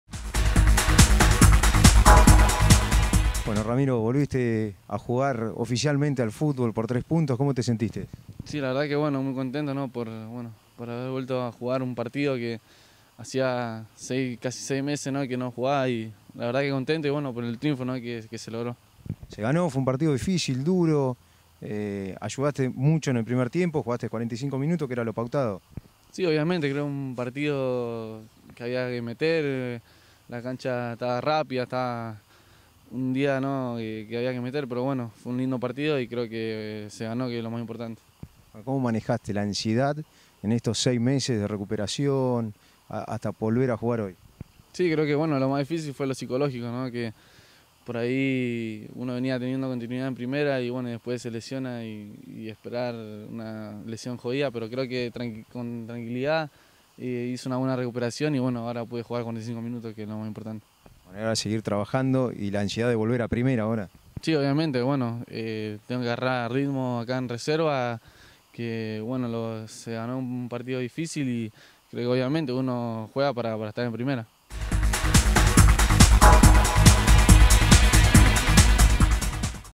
Ramiro Funes Mori jugó en Reserva
El defensor regresó a las canchas tras la lesión que le demandó más de seis meses de recuperación. Lo hizo en la victoria por 2 a 1, ante Unión.